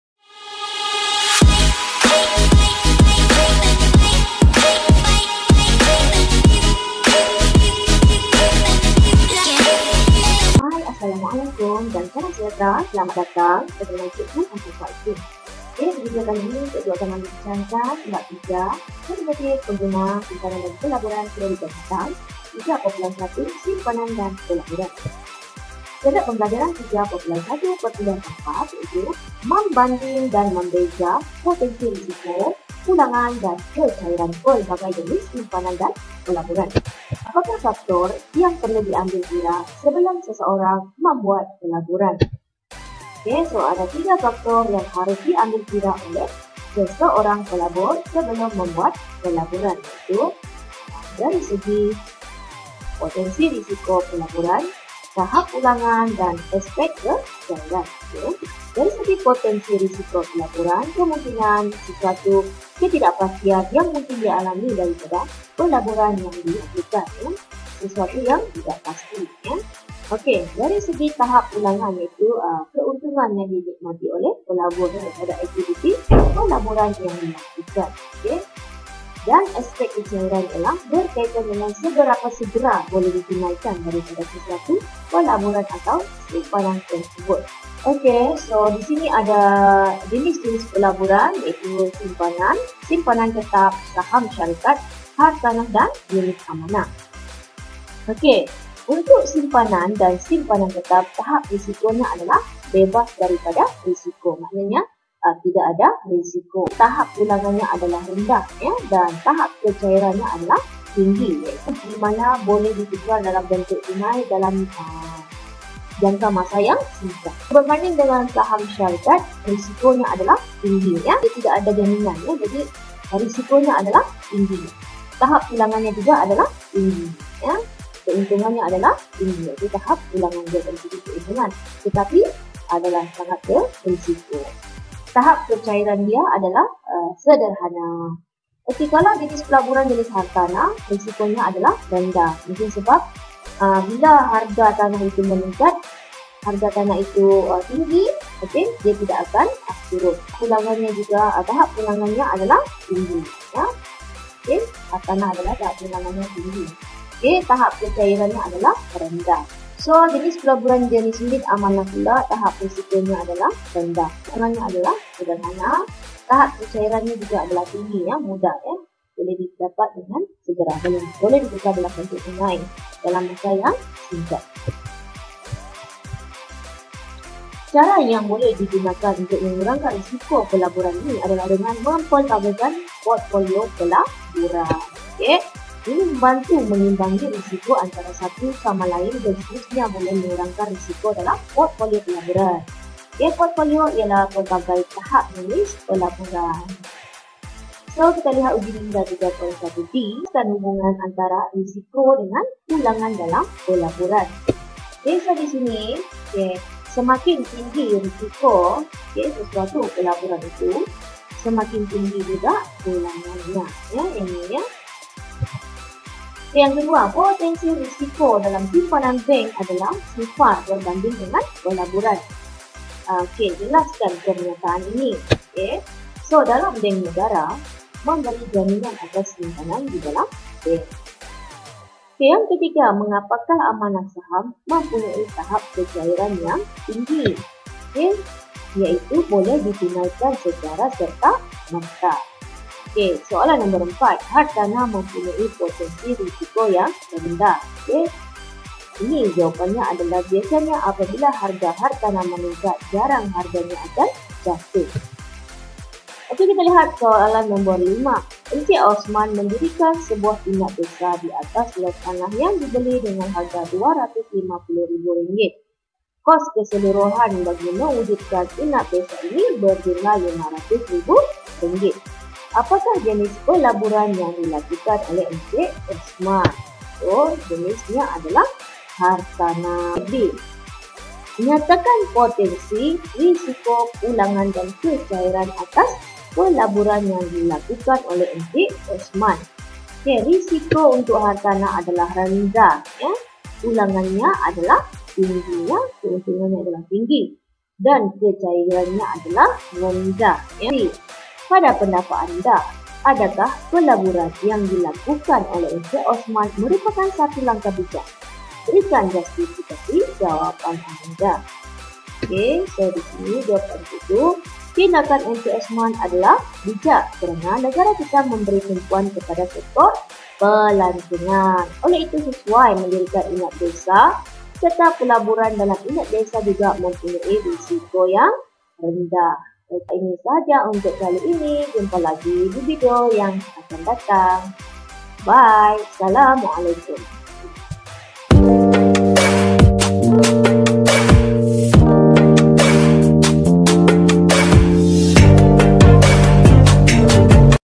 3.1.4 RISIKO,PULANGAN DAN KECAIRAN
Video ini membincangkan tiga faktor yang harus diambil kira oleh seseorang pelabur sebelum membuat pelaburan iaitu:
1. Risiko pelaburan - Terdapat ketidakpastian atau tidak ada jaminan tentang keuntungan.

2. Tahap pulangan - Keuntungan yang diperoleh daripada aktiviti pelaburan. Untuk mendapat pulangan tinggi,tahap risiko juga tinggi.

3.Aspek kecairan - Keadaan dimana aset-aset pelaburan boleh ditukar dalam bentuk tunai dalam jangka masa yang singkat.

Terdapat pelbagai simpanan dan pelaburan iaitu simpanan ,simpanan tetap,saham,hartanah dan unit amanah yang mempunyai tahap risiko,tahap pulangan dan tahap kecairan masing-masing.

Semoga bermanfaat🌹❤️